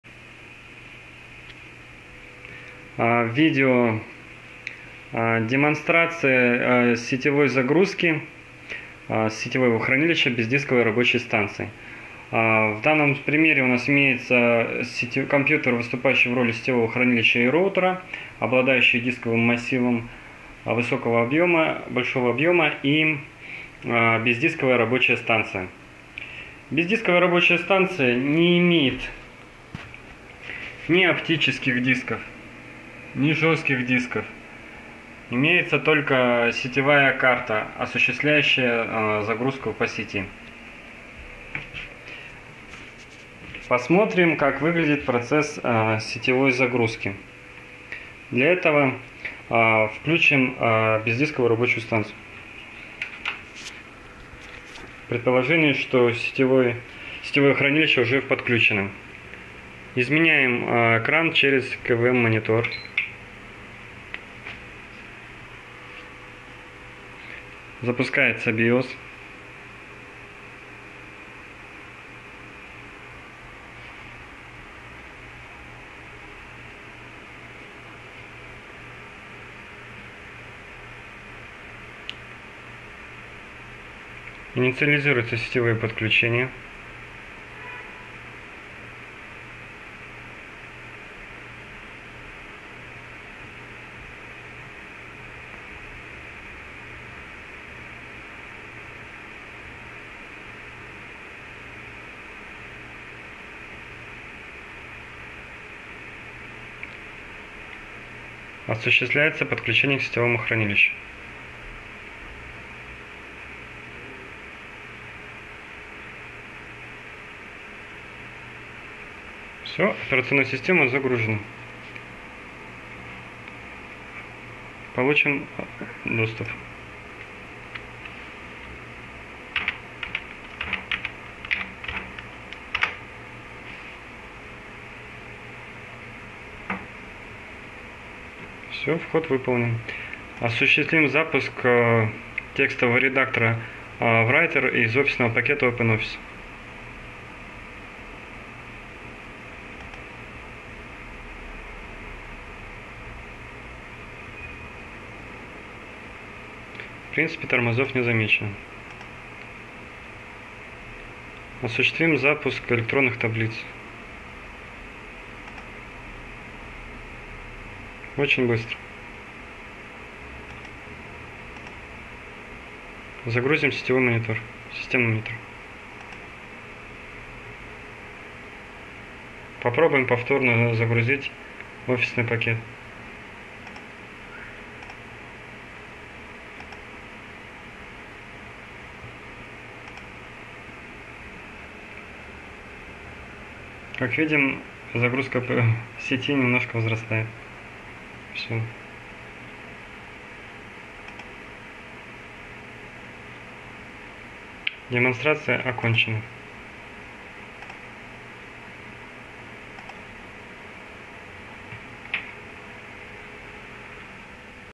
Сетевая загрузка Ubuntu
Сетевая загрузка рабочей станции Ubuntu 10.04 с использованием iSCSI и gPXE.